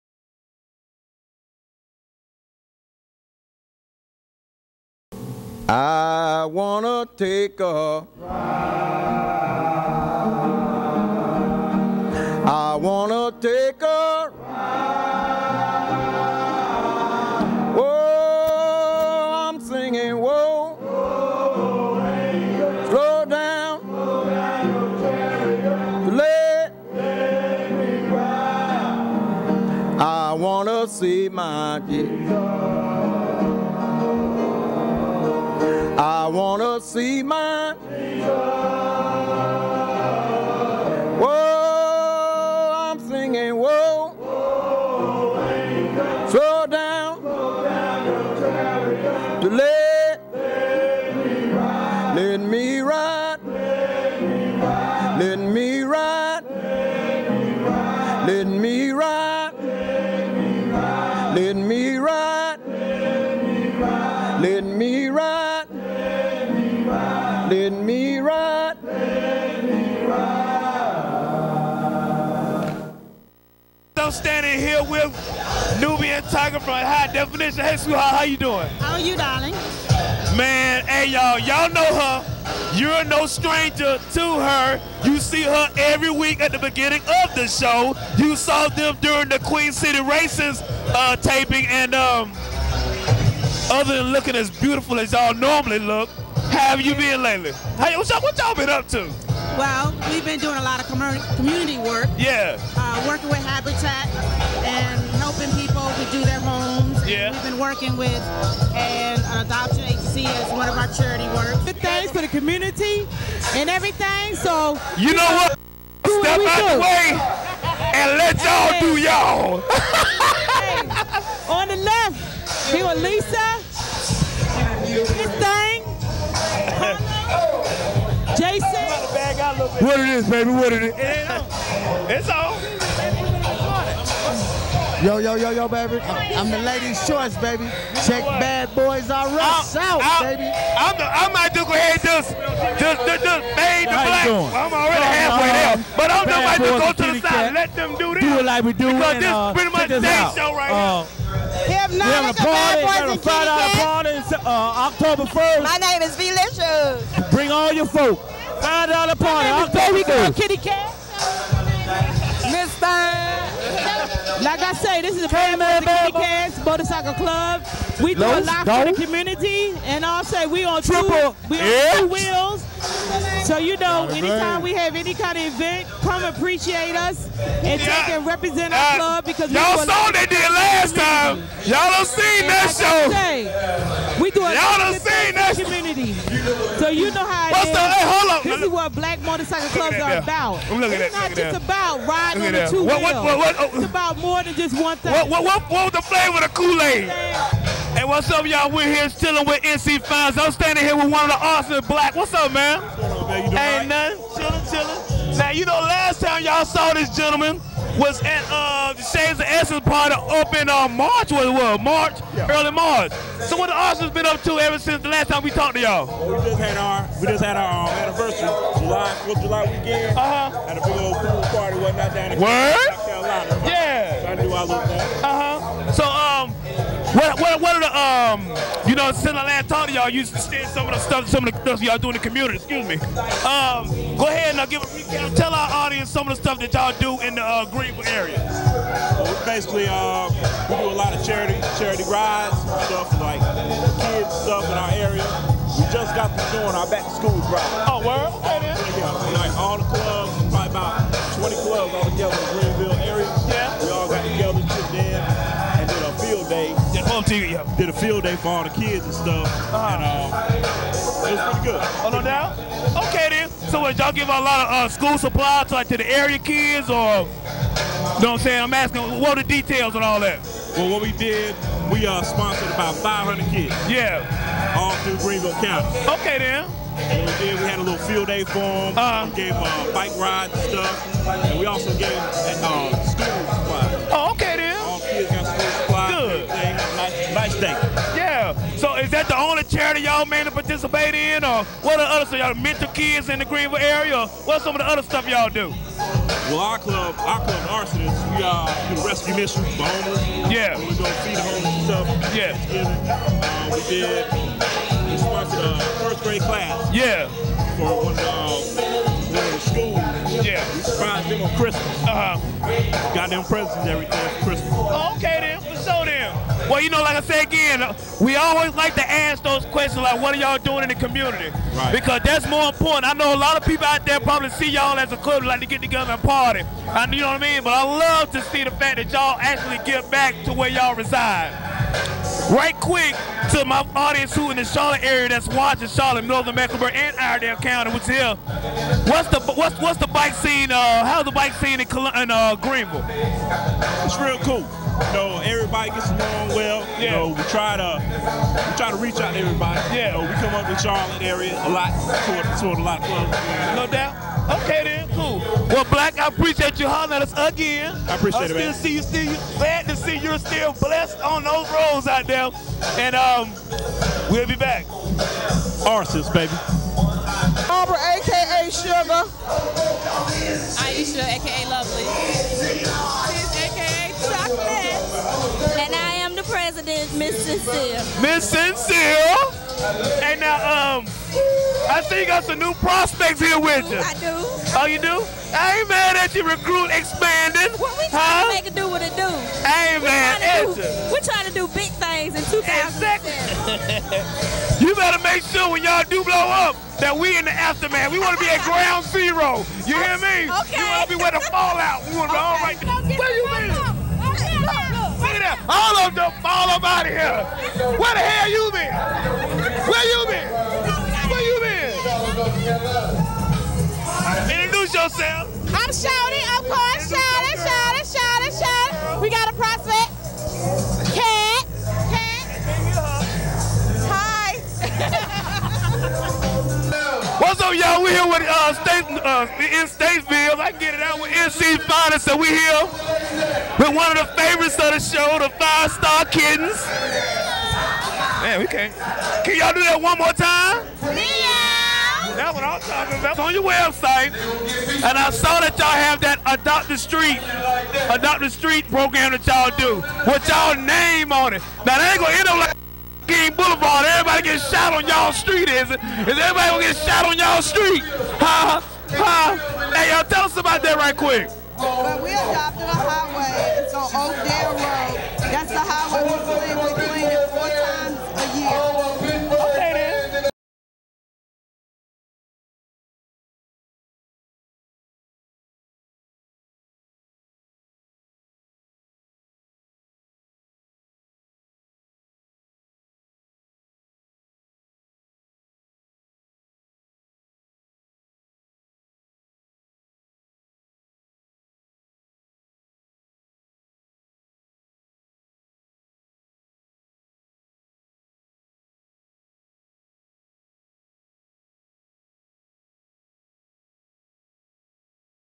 Cruise Chronicles TV:#2 NC Finest 2nd Anniv. Party
Cruise Chronicles is a tv show that celebrates the motor vehicle and the people that pilot them. as they celebrate their 2nd anniversary.